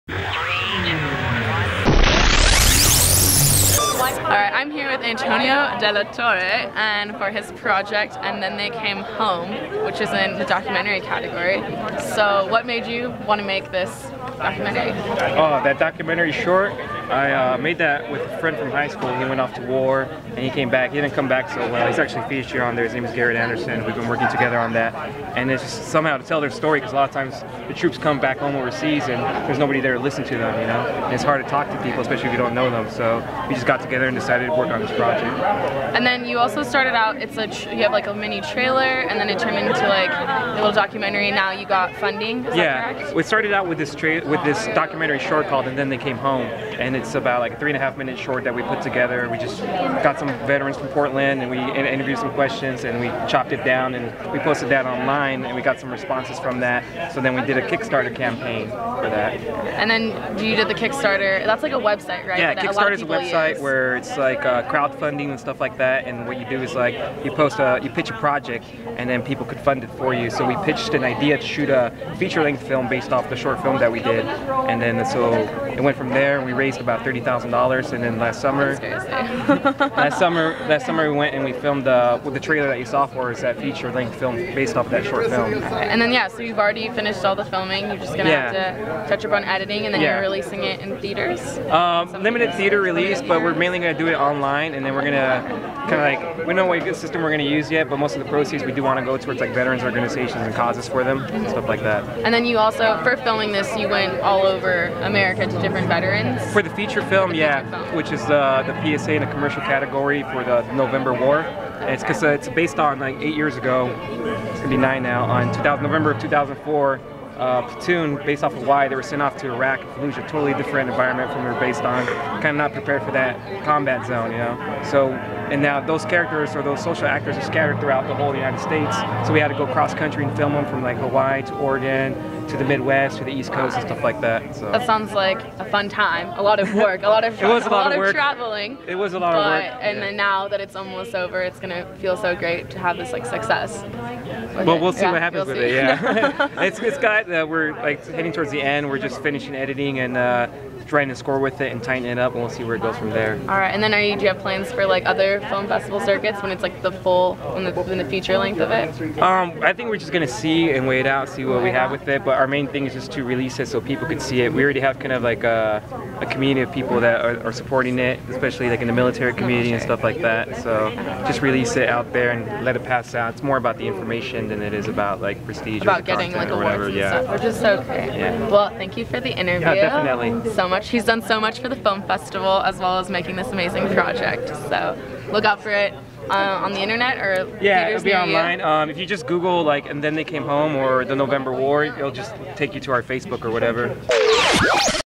Interview w/ Antonio de la Torre at CSULA Golden Eagle Film Festival 2013
Golden Eagle Radio Correspondent Skylar Bowman interviewed Director Antonio de la Torre at Cal State LA's 16th Annual Golden Eagle Film Festival.

Antonio's submissions were "And Then They Came Home" for the Category of Documentary and "November War" for PSA/ Commercial.